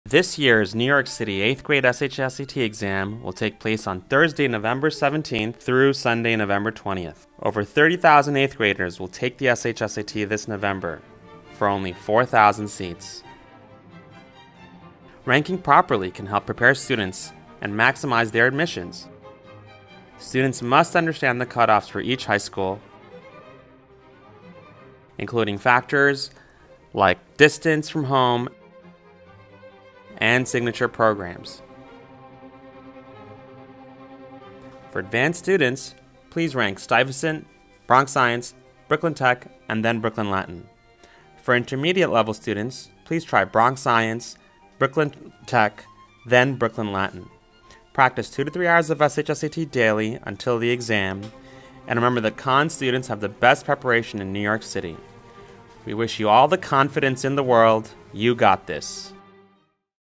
New York City SHSAT School Rankings
Students! The 2022 SHSAT Exam is rapidly approaching and you need to make sure you are fully prepared. 📝💻

This year the SHSAT will be on Thursday November 17th for registered DOE 8th graders and run through the 20th for other students. 🗓️📆

Everyone knows about preparing for the test itself, but students should also know about the rankings of the Specialized High Schools. 1️⃣2️⃣3️⃣4️⃣

Not only should you know this for the test itself, as you are required to ranking the schools in order of which one you want to go to the most. 😍🤔💗

But you should also know this because of factors such as distance and special programs. 🗺️📍⏰

Please take a look at our short to better learn about the rankings and good luck to the Khan’s students! 👩‍🏫📚🚌